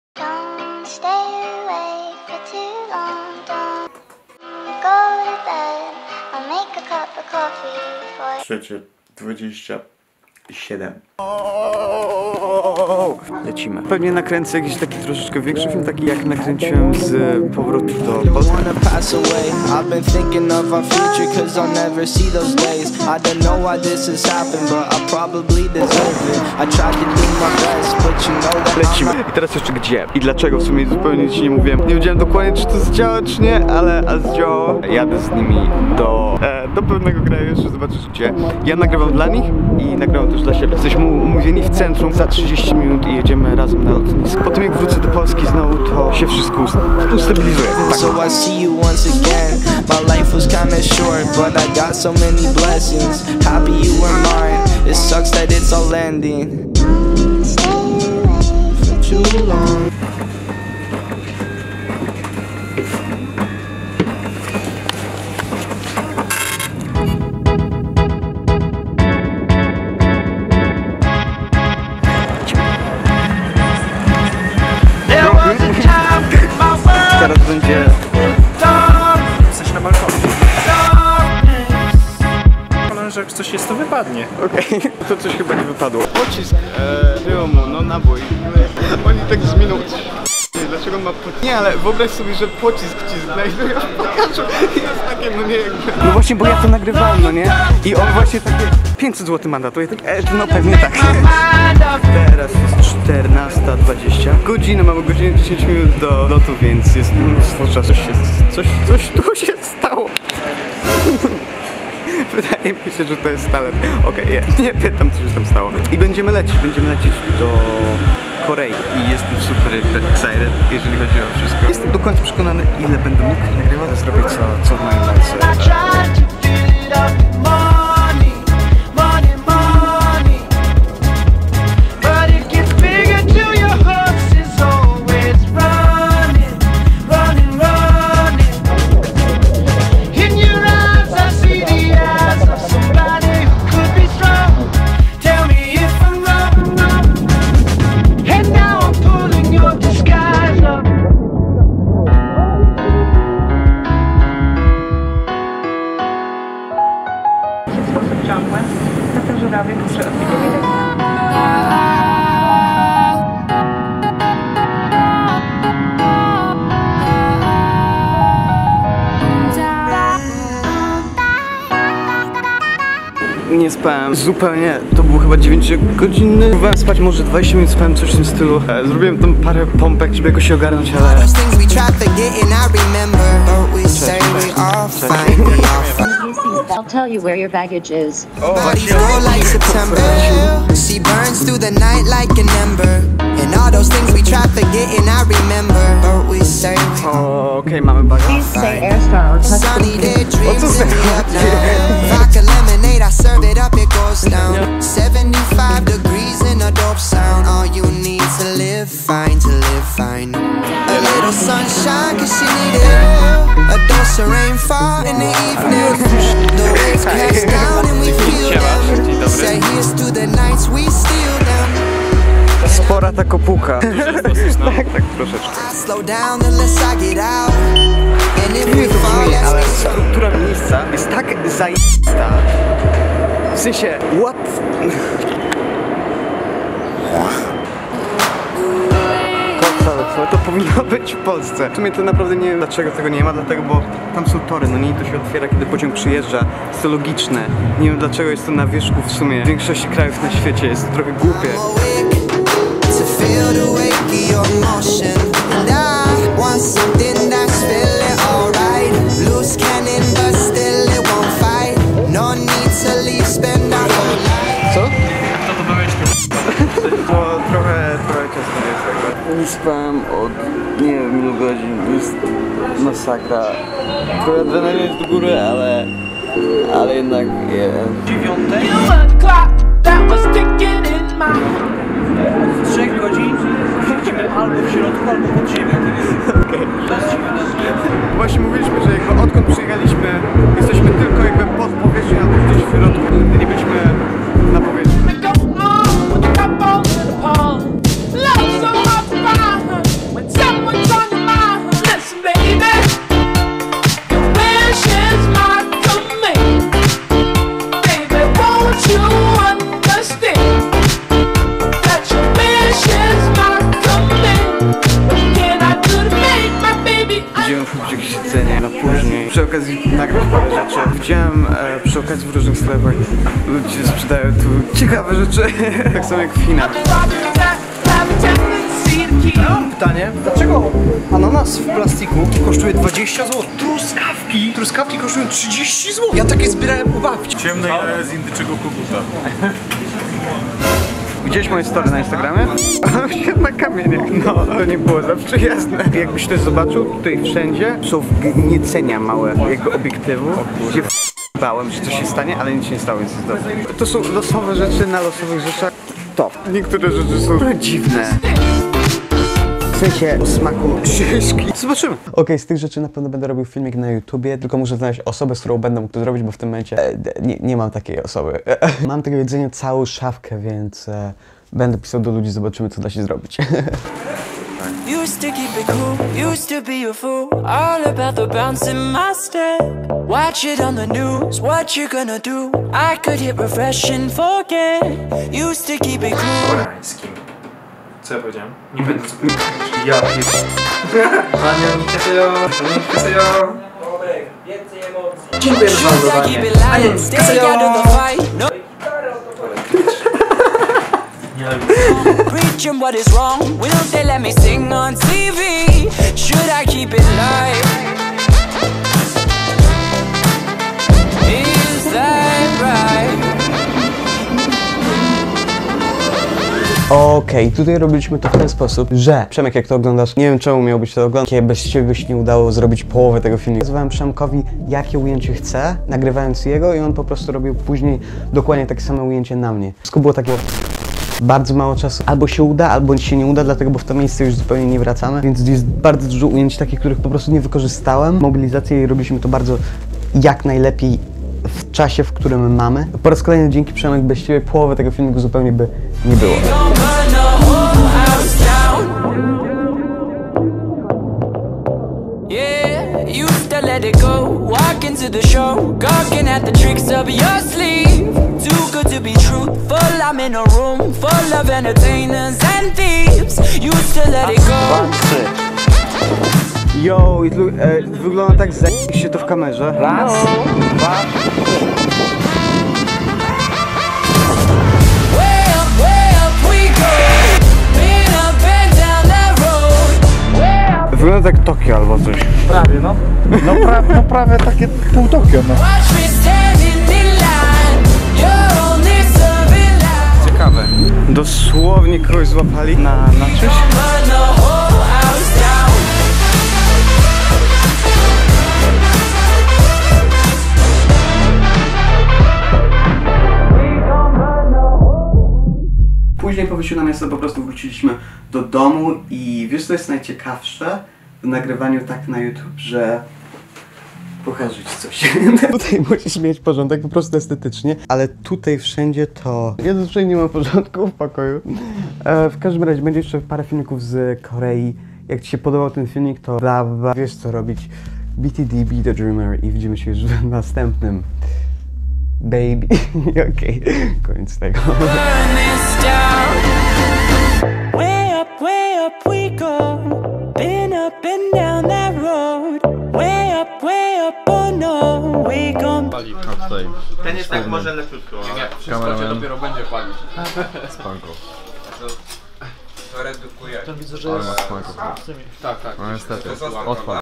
PRZEMYCIŁ NABÓJ NA LOTNISKU?! KOREA - 1
Dzisiaj wrzucam pierwszy vlog z Korei, parę ich jeszcze zrobiłem, mam nadzieję że poprawi Ci chociaż trochę humor podczas tego meeeeega dziwnego okresu 😃😂

Wieeeecej na..

Why do you still looking at the description? (o - O)]